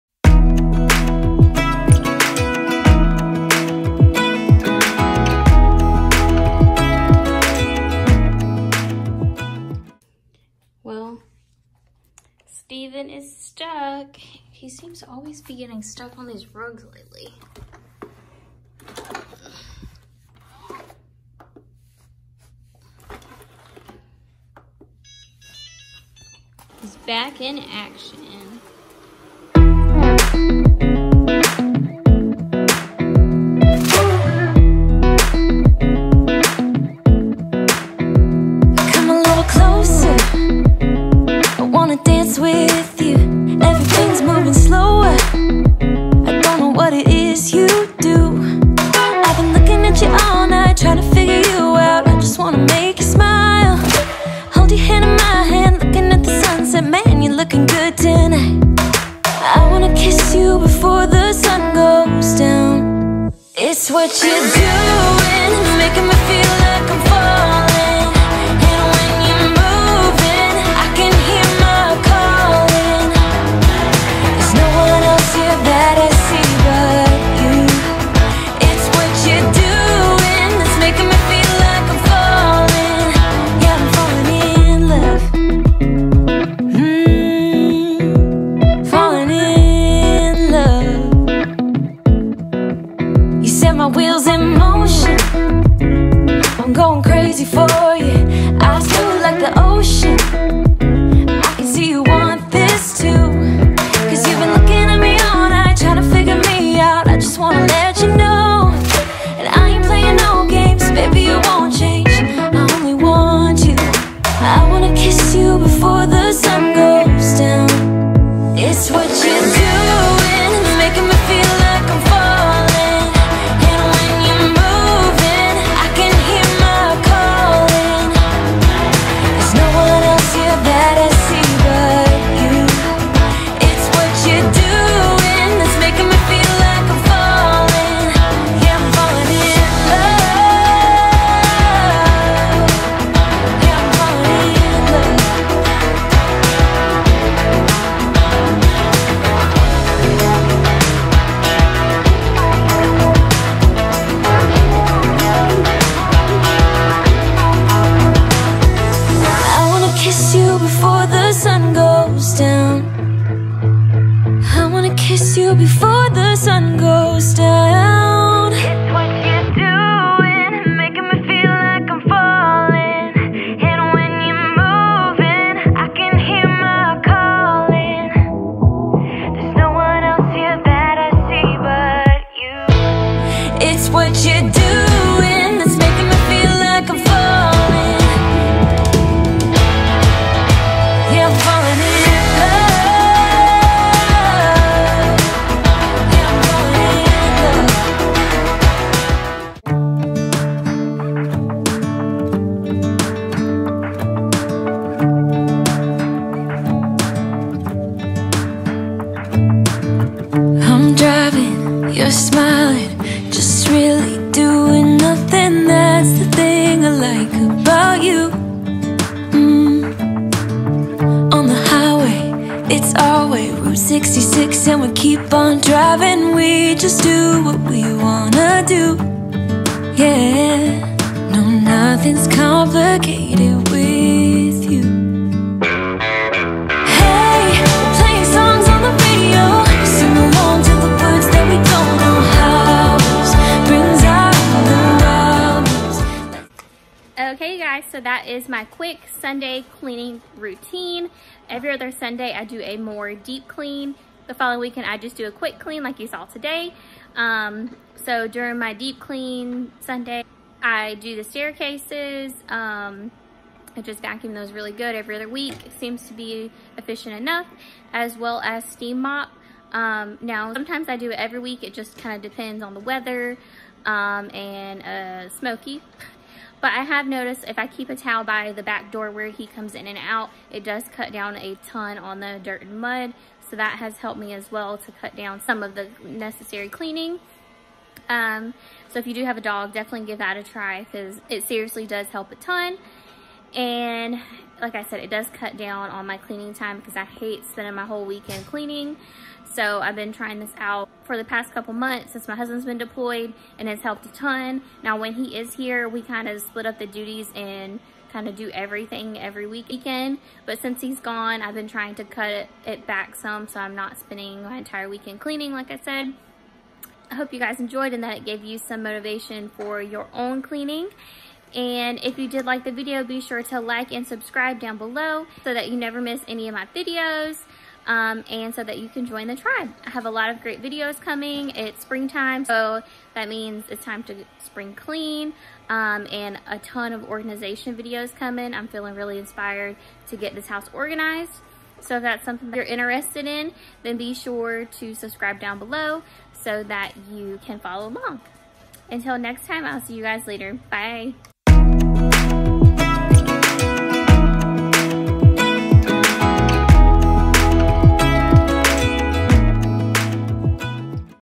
Sunday Quick Clean With Me
Follow along as I do my weekly SUNDAY Quick Cleaning! Cleaning motivation to get your home cleaned quickly after a long week!